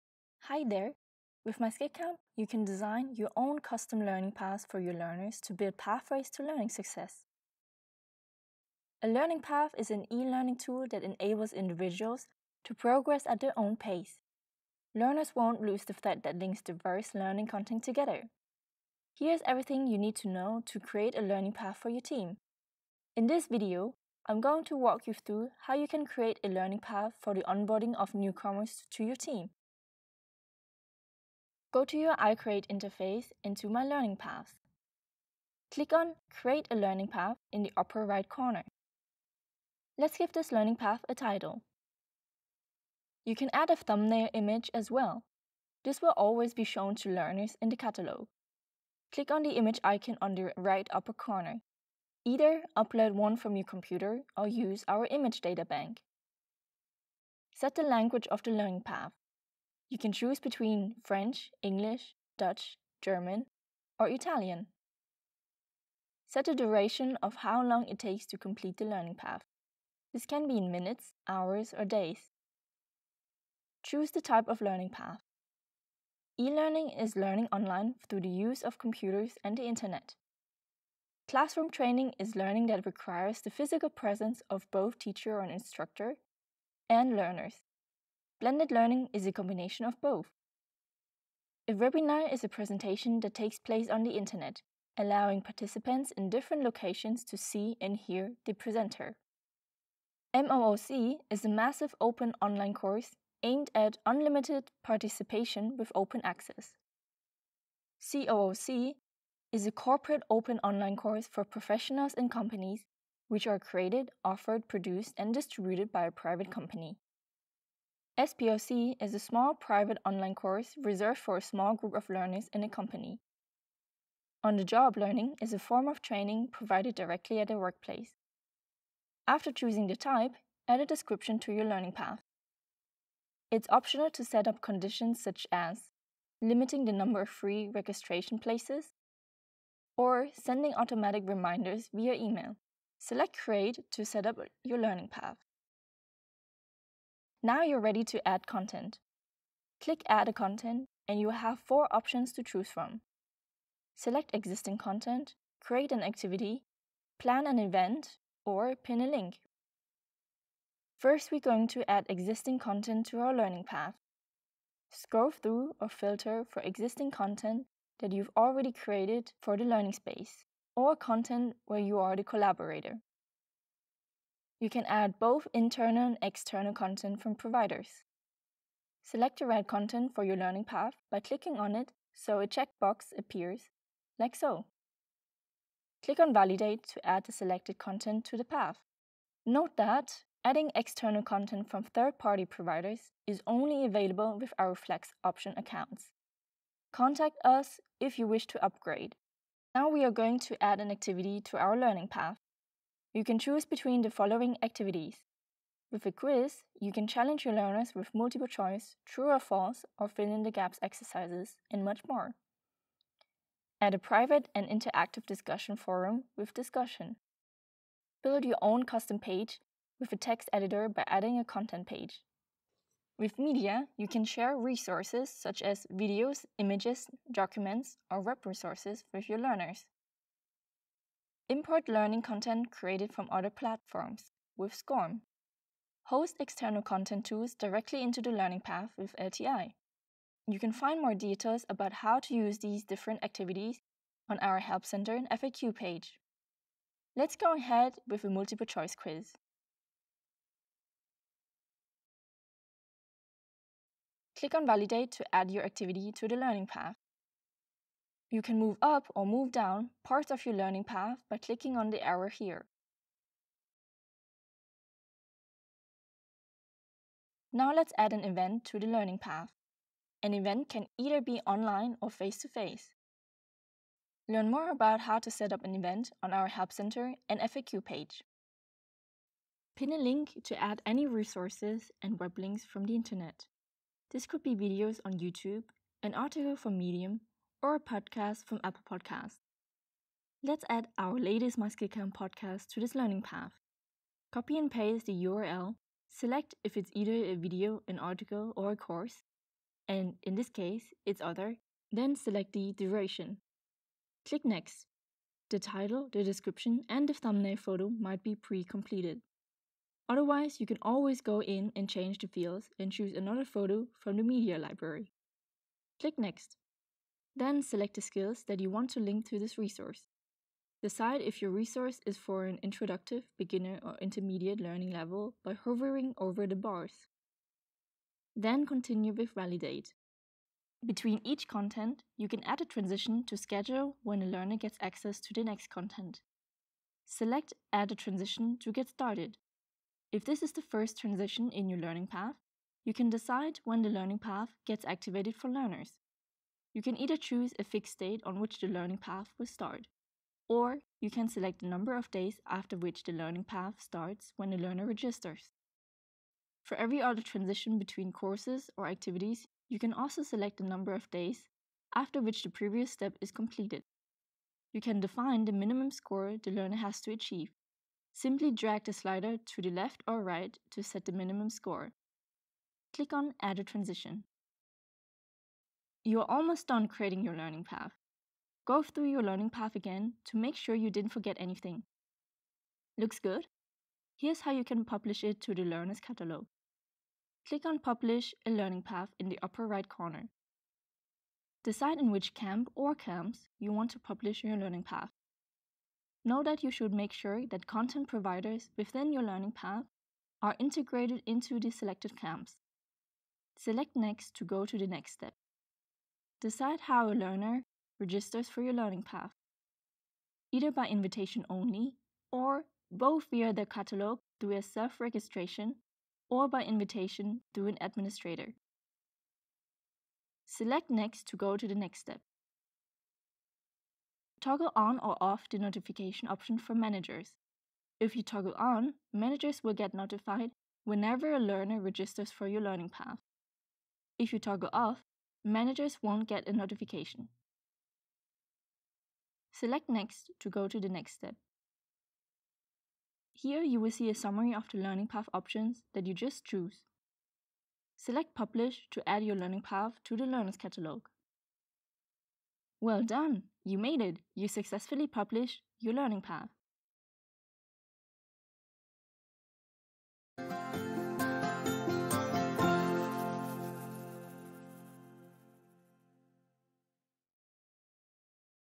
How do I create a learning path: myskillcamp
Build a complete learning journey for your employees/learners instead of setting a single course.
We'll show you how to add content easily using the myskillcamp platform.

Timestamps
00:00 Intro
00:32 How to set up a learning path
02:24 Adding content and activities
05:30 Adding transitions between courses
06:10 Publishing your learning path
07:26 Outro